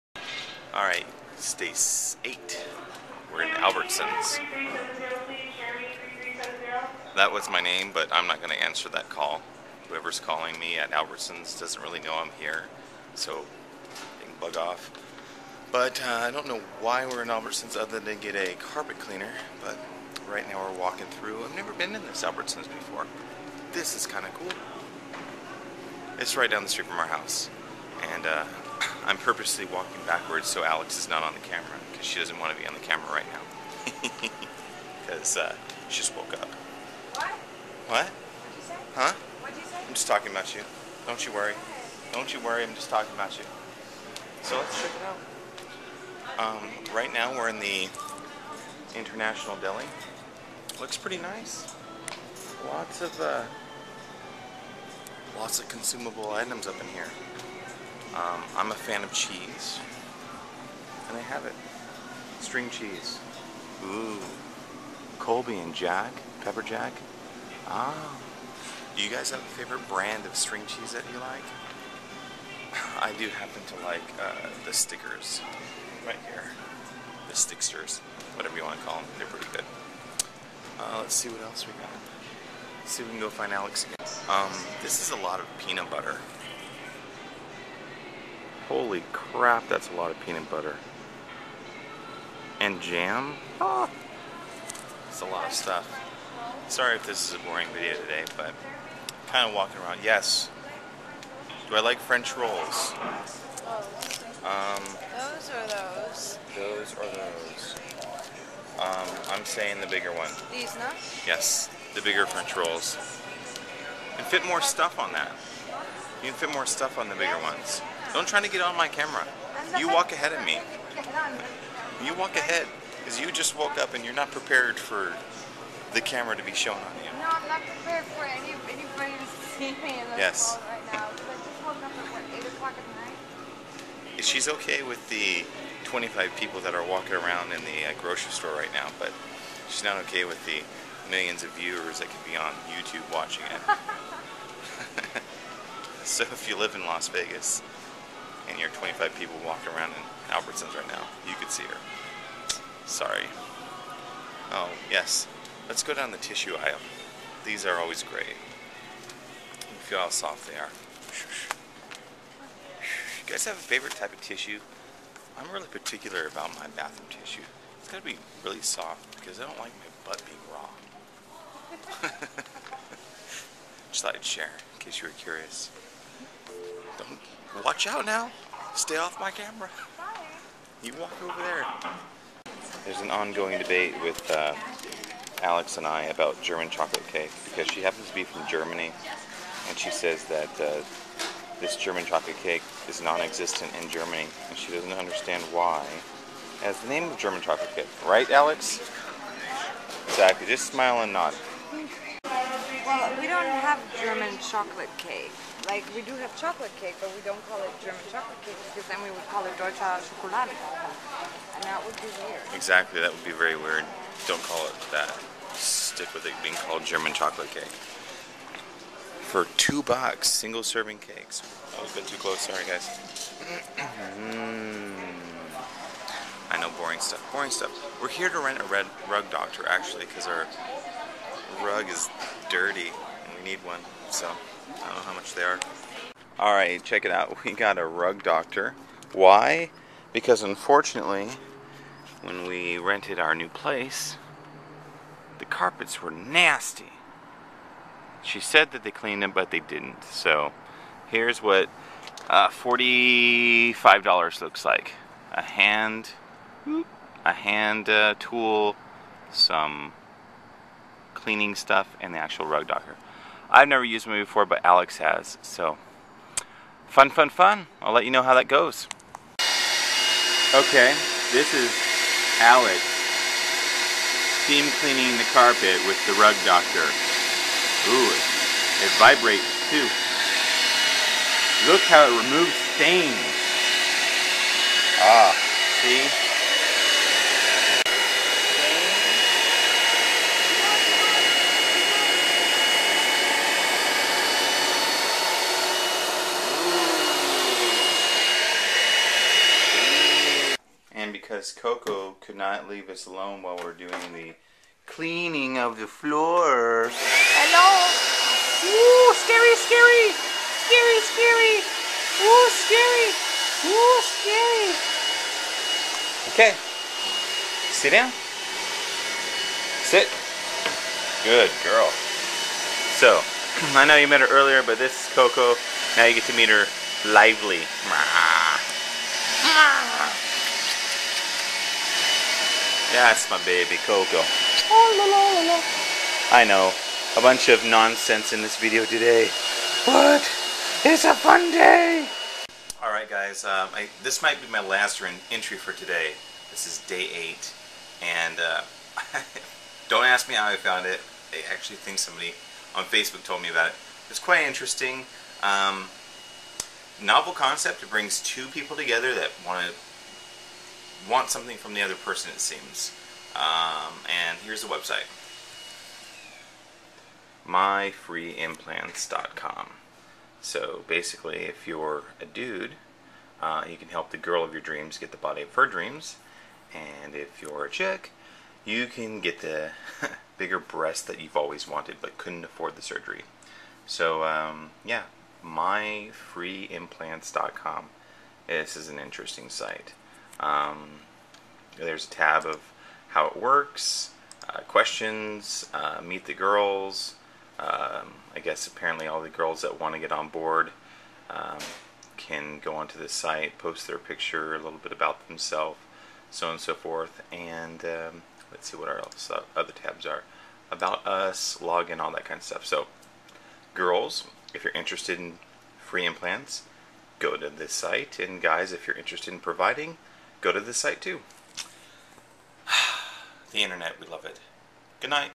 My FREE Implants... What?!? (8/3/09 - Day 8)
Well, apparently there is a site that makes it easier for women who want breast implants and the guys willing to fork over the money for those women to get breast implants, to more easily meet up with one another.

I guess it would be kind of awkward to just bring the subject up to a random guy on the street, "Hey dude, I want breast implants, would you buy them for me?"  Therefore, having a site for this situation to seem less awkward, seems quite appropriate.  hahaha  What do you think?